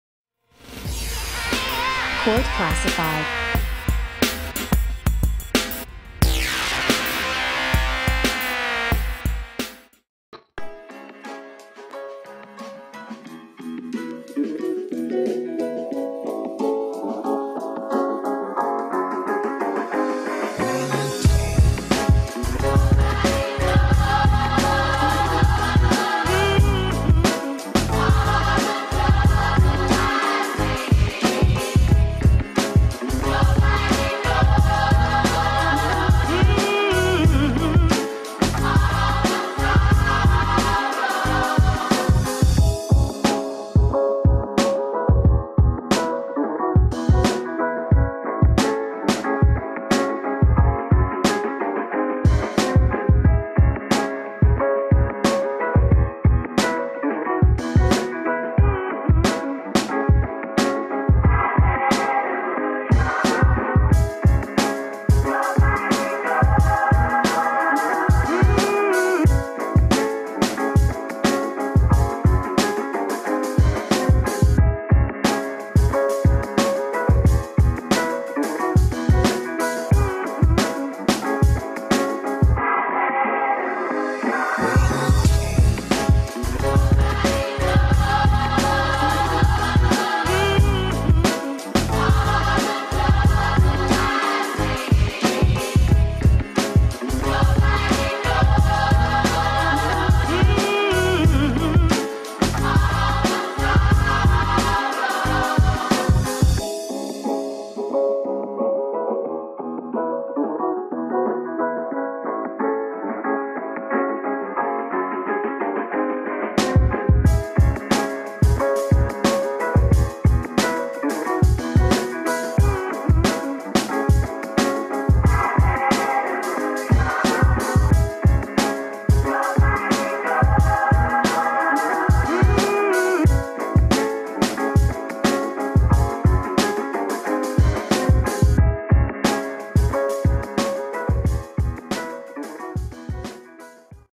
CourtClassified: Rasheed Johnson (DePaul HS '20) MSU Prospect Camp Mix
C o u r t C l a s s i f i e d . c o m